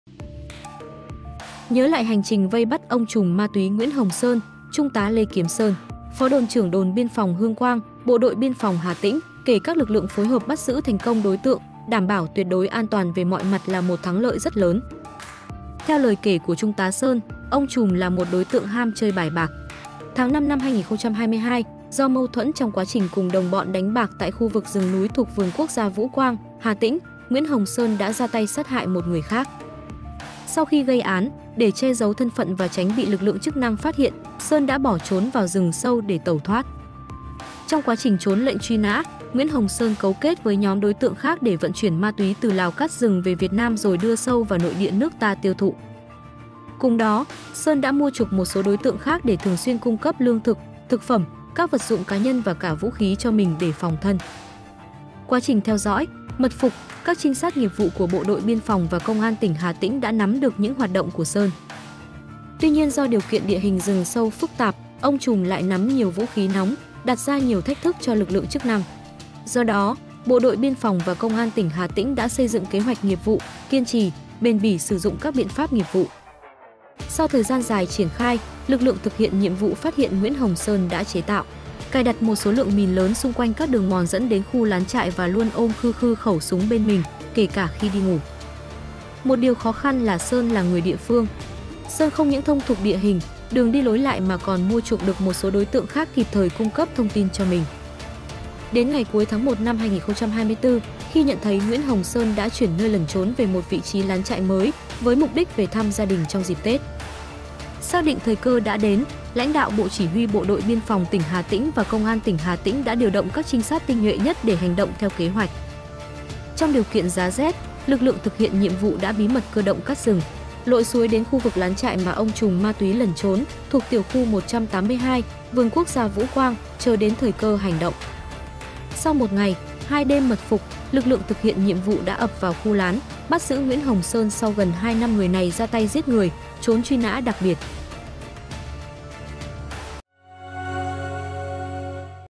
Hành trình vây bắt trùm ma túy Nguyễn Hồng Sơn giữa rừng sâu, được cài mìn bảo vệ
Quá trình vây bắt, 'ông trùm' ma túy Nguyễn Hồng Sơn đã chế tạo, cài đặt một số lượng mìn lớn tại những đường mòn dẫn đến khu lán trại trong rừng sâu đang lẩn trốn và luôn ôm khư khư khẩu súng bên mình, kể cả khi đi ngủ.